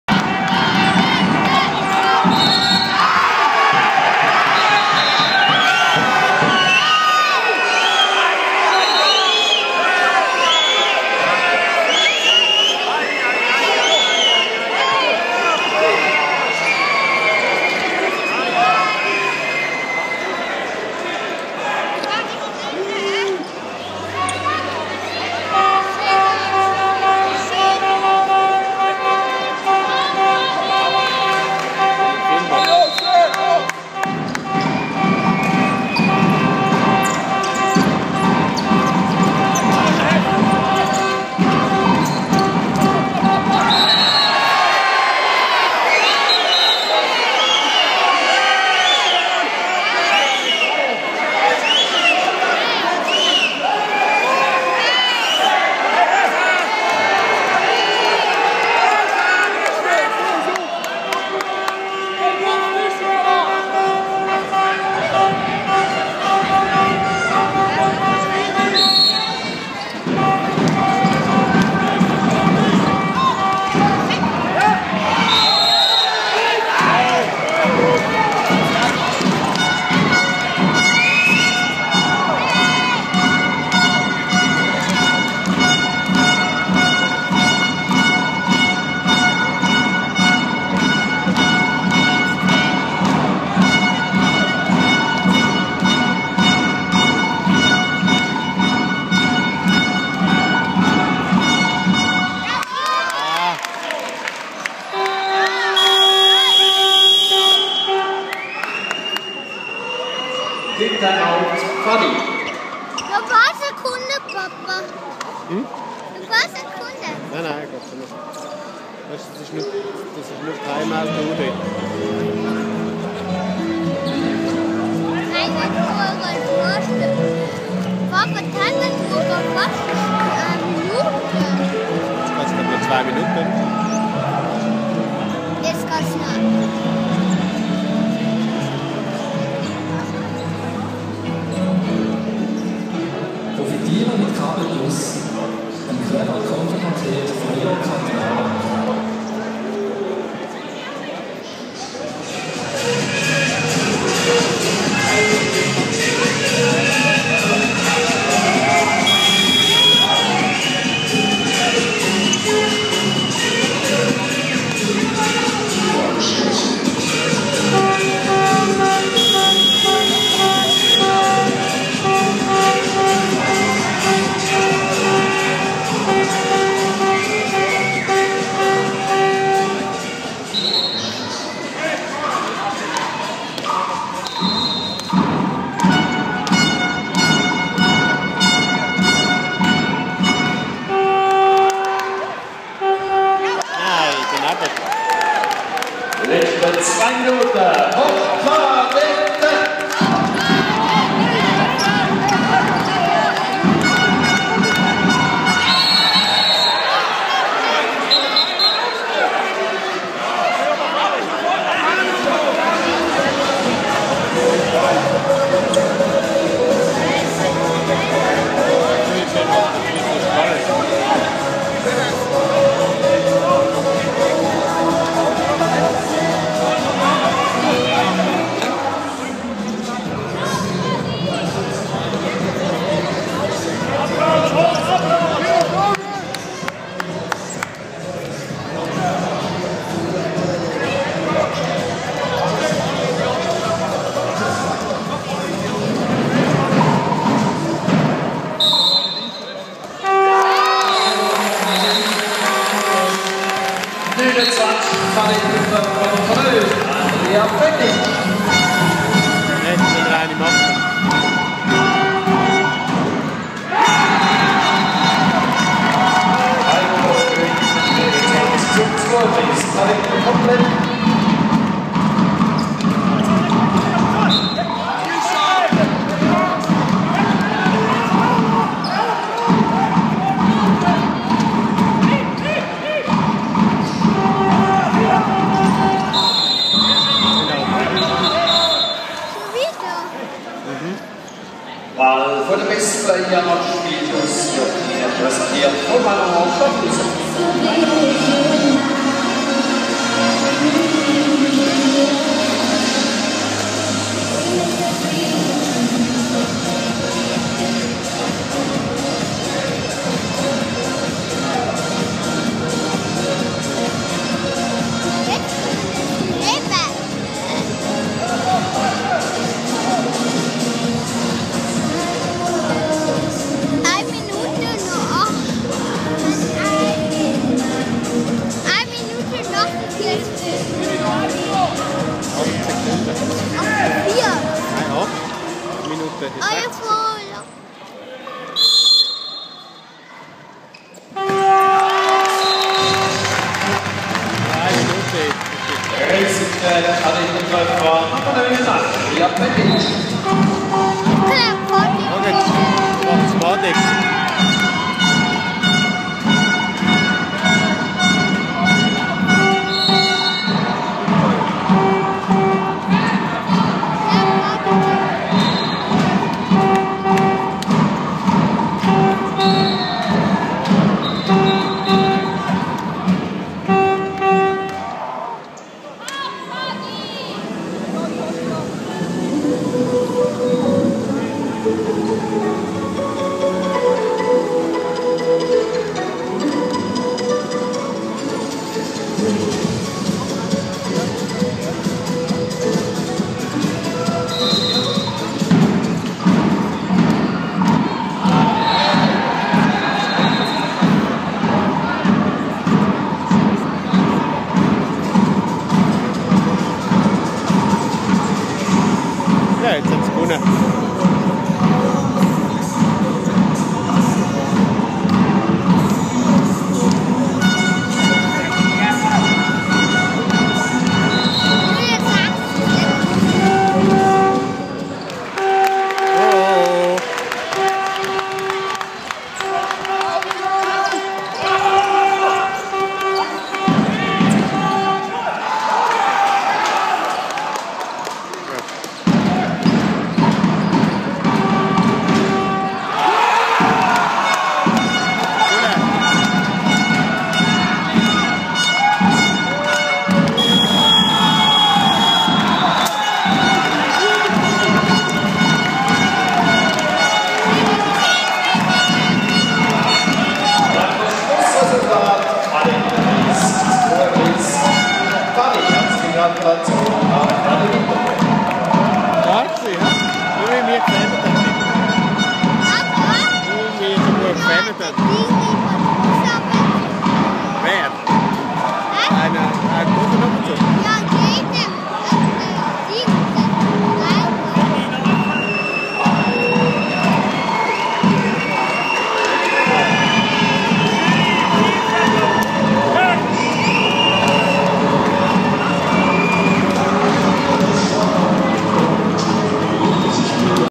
Letzte Minuten: Kadetten - Pfadi am 22.12.
Pfadi schlägt die Kadetten verdient mit 32:30!